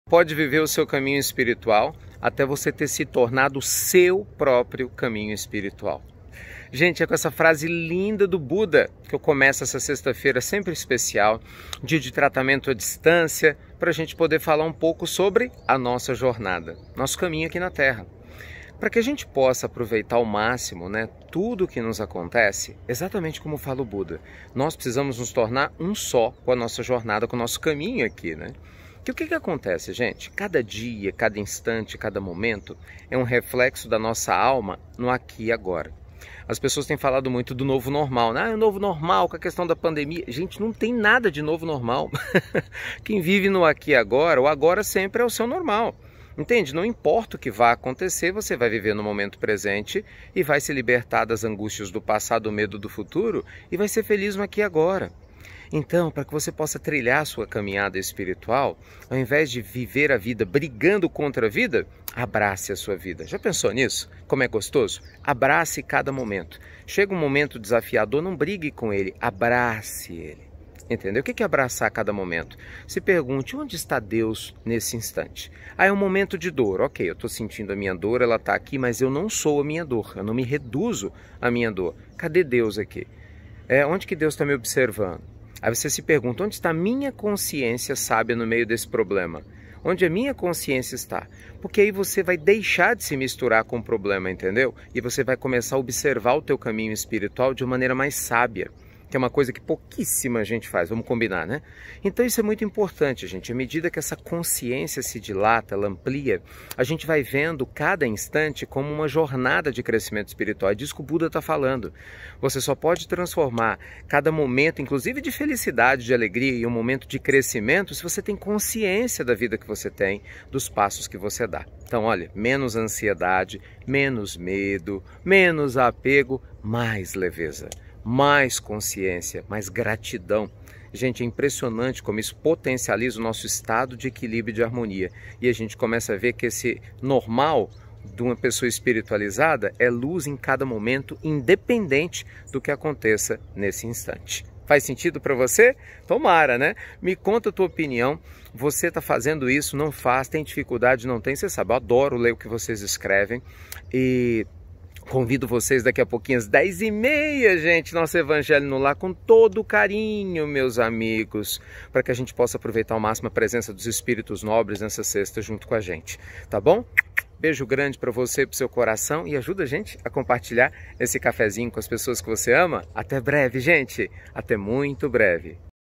CAFÉ ☕ COM RICARDO: Torne-se seu Próprio Caminho Espiritual...🙏
"Você só pode trilhar seu caminho espiritual ao se tornar ele próprio..." BUDA
Já imaginou uma vida pacífica, serena, completamente em paz?
Mesmo nas dores, sentir a alegria da gratidão e leveza em sua alma?🤔
No CAFÉ ☕ COM RICARDO vamos falar sobre essa possibilidade REAL, para quem compreende algumas Leis Divinas...
▪︎ Ajude-nos a COMPARTILHAR essa MENSAGEM com quem você AMA!🥰🙏